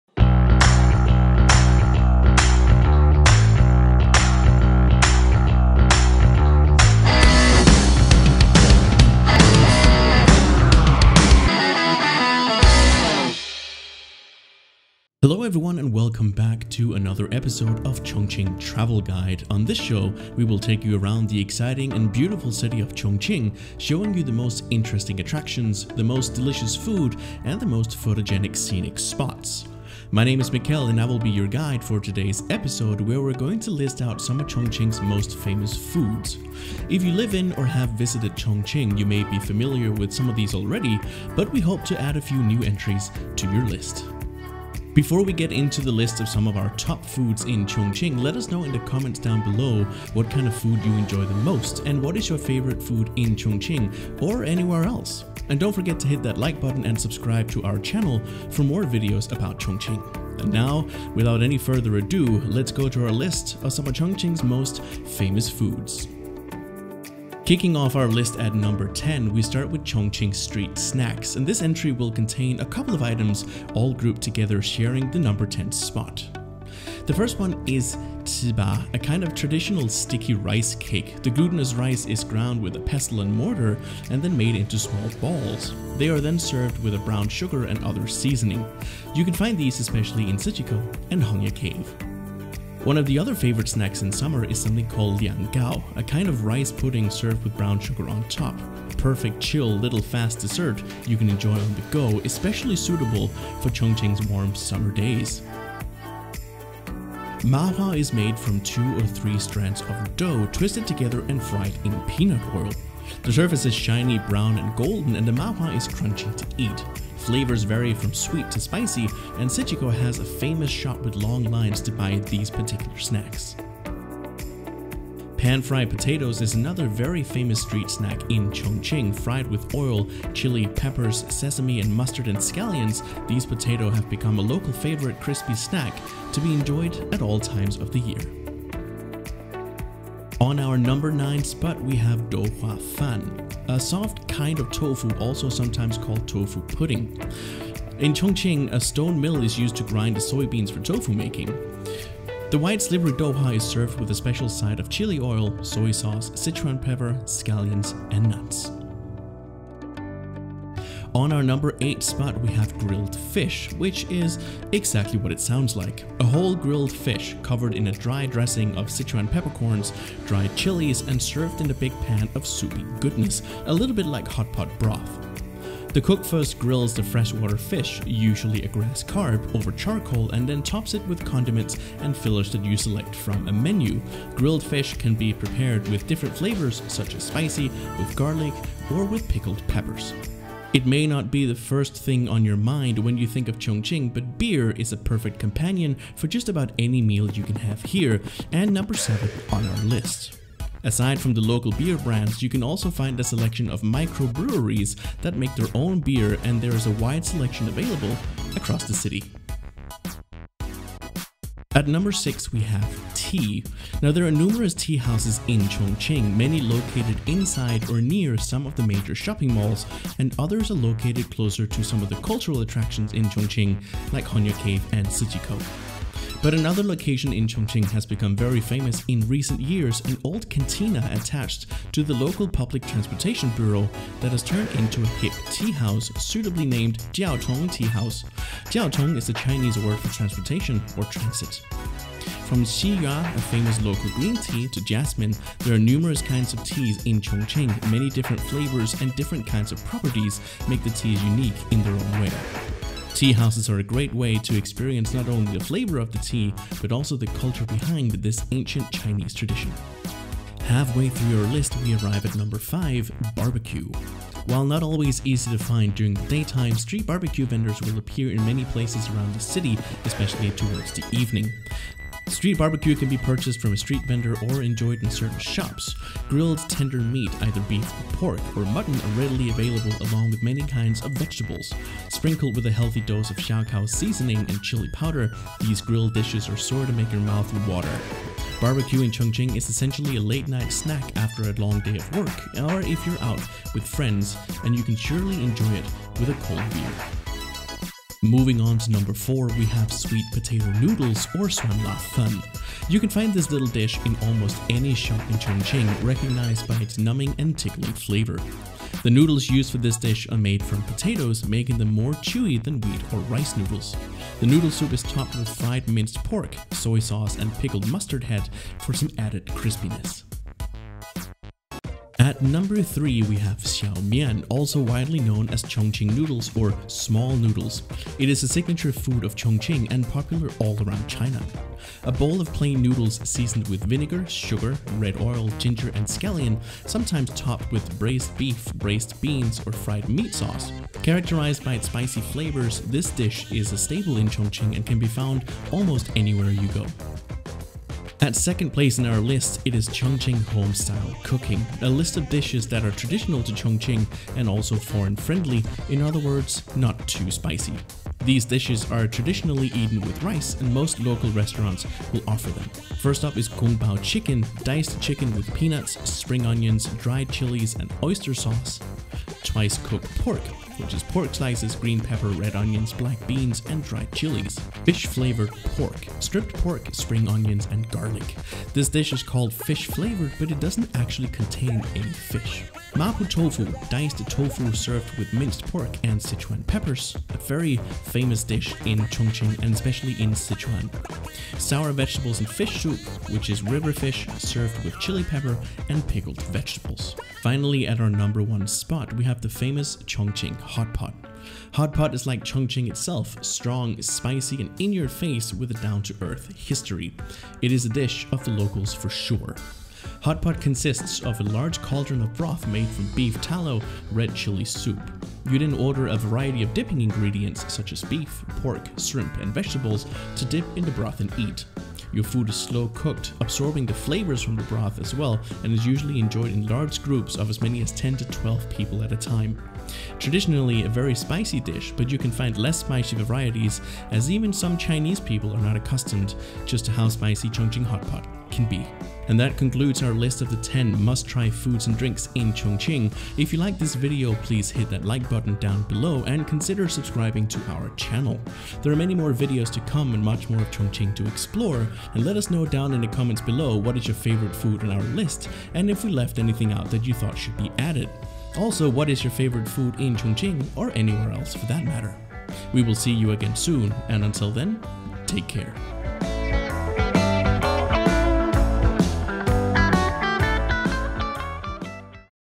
Top 10 foods to Eat in Chongqing | Chongqing Travel Guide
When we are on vacation, one of our favorite things to do is eat. And in Chongqing, you shall never miss so many delicious food. To help you choose, we get out our foodie friends and local Chongqing people to help us create the inspiring list of top 10 things to eat when you are in Chongqing.